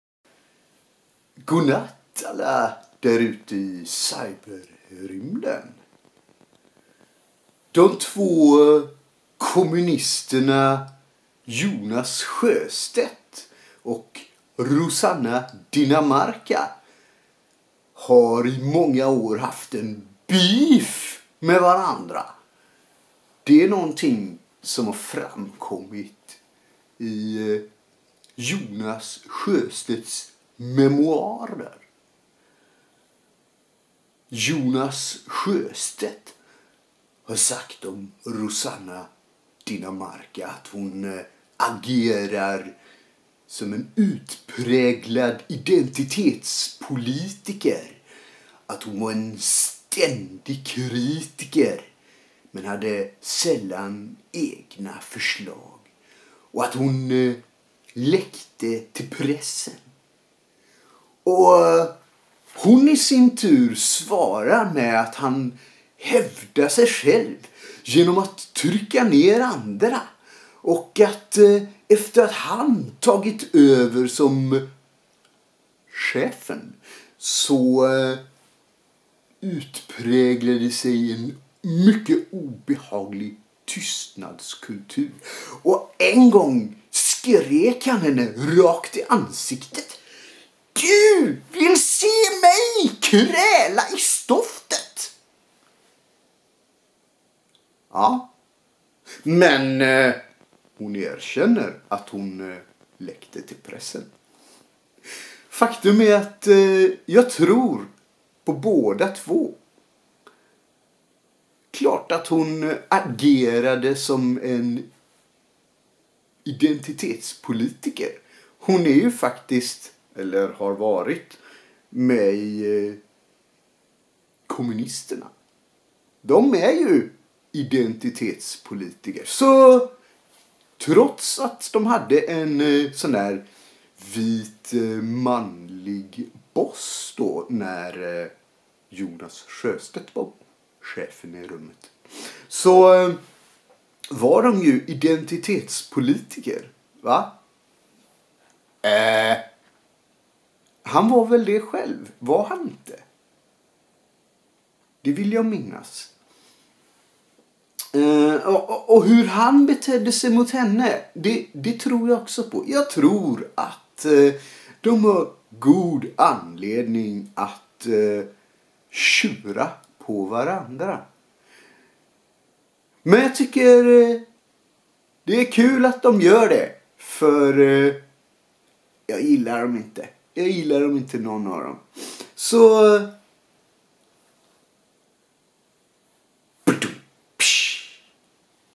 Jonas Sjöstedt i bråk med Rossana Dinamarca
Vill du stödja mitt arbete? Swish: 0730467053

Att kommunisternas före detta riksdagsledamot Rossana Dinamarca ständigt var i bråk med Sverigedemokraterna kan ju knappast ha undgått någon. Det var ju hon som gick omkring i en t-shirt med texten "SD=rasister" och som nedlåtande fnyste åt dåvarande andra vice talman Björn Söder att han inte var hennes talman- också det på grund av att han var sverigedemokrat. Dinamarca ville gärna sitta i riksdagen och håva in en fet månadslön, men som kommunist kunde hon förstås aldrig visa respekt för de väljare som riksdagsledamöterna ska representera.

Men att hon dessutom bittert såg sin egen dåvarande partiledare Jonas Sjöstedt som en ren nemesis, det var mer än iallafall jag lyckats snappa upp. Den saken tror jag de lyckats hålla för sig själva i maktens korridorer.

Fram tills helt nyligen då när Sjöstedt kom ut med sina memoarer "Allt kommer att bli bra", där han bestämt sig för att gå till angrepp mot sin tidigare ärkefiende, och på så vis vara den som skrattar sist i deras långa och bittra strid.

En rolig historia, tycker jag! Det har alltid verkat som om Jonas Sjöstedt varit så oerhört omtyckt- trots att han var en vit, kränkt, någorlunda övre medelålders och heterosexuell cis-man som ledare för ett parti som formligen HATADE sådana- och det har alltid stört och irriterat mig. Men nu vet jag iallafall att inte ALLA kommunisterna älskade honom...